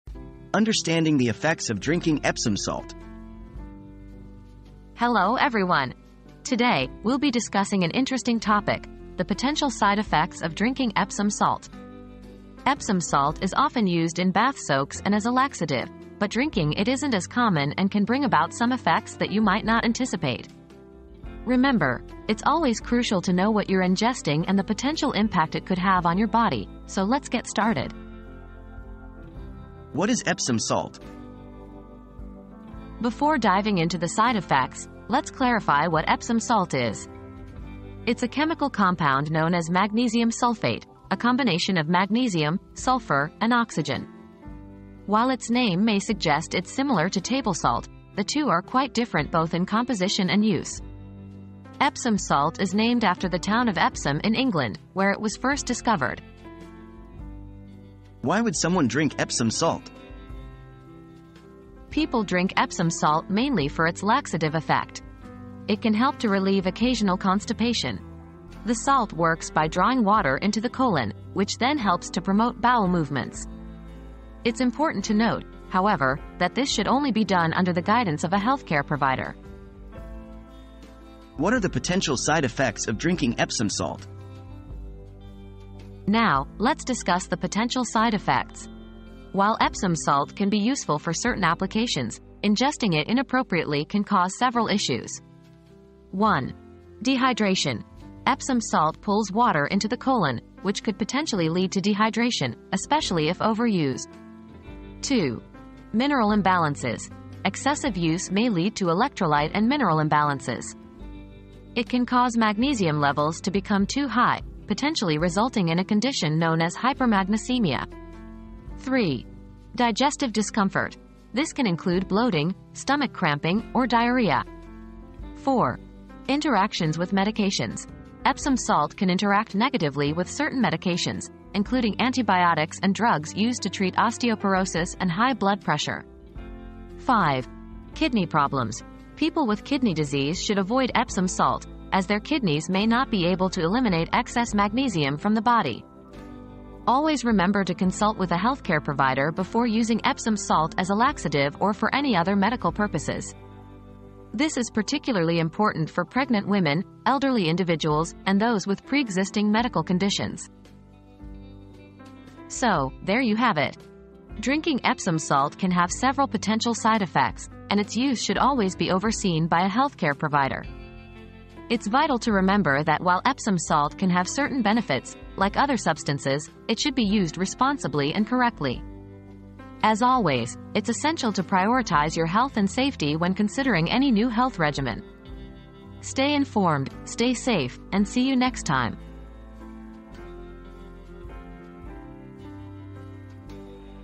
Understanding the Effects of Drinking Epsom Salt
What are the potential side-effects of drinking Epsom salt? • Discover the potential side-effects of drinking Epsom salt in this informative and eye-opening video. From dehydration to mineral imbalances, learn why ingesting Epsom salt can have unexpected consequences on your body. Join us as we delve into the science behind this chemical compound and explore the risks associated with its consumption. Stay informed and prioritize your health!

00:00 • Understanding the Effects of Drinking Epsom Salt
00:29 • What is Epsom Salt?
00:58 • Why Would Someone Drink Epsom Salt?
01:21 • What Are the Potential Side-Effects of Drinking Epsom Salt?